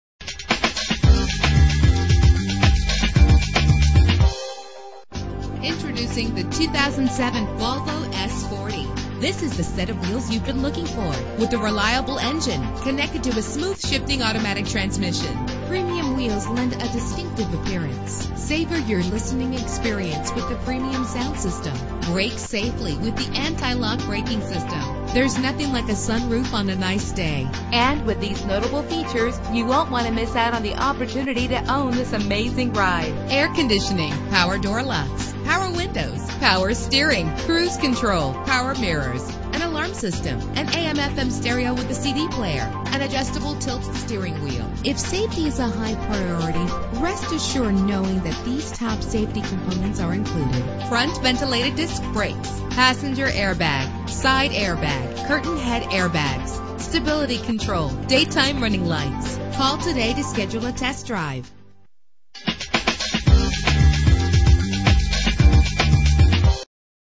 2007 Volvo S40 - Salt Lake City UT - NATE WADE SUBARU
Year: 2007
Make: Volvo
Model: S40
Trim: 2.4i
Engine: 2.4 liter inline 5 cylinder 20 valve
Transmission: Automatic
Color: Passion Red
Mileage: 57783
Address:
1207 S Main St